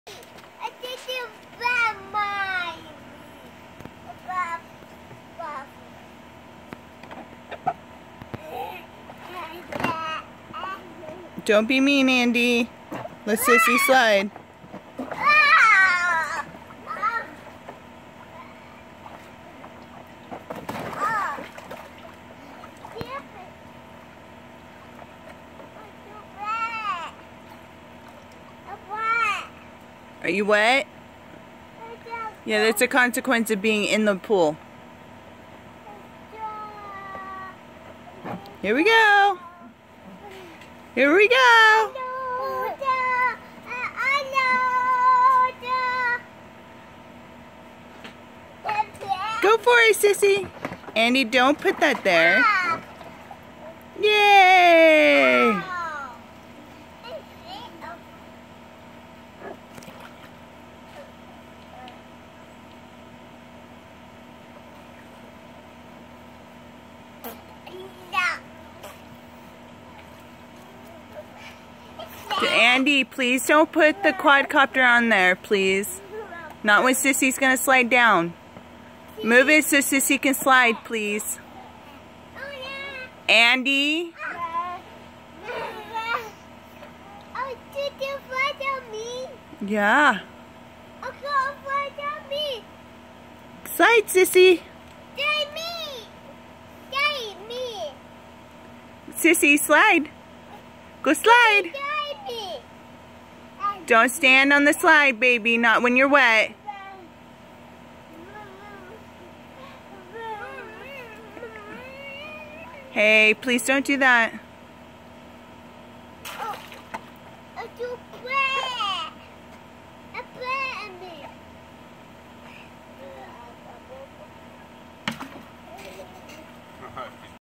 Toddler's summer fun
Andy and Amelia playing in the pool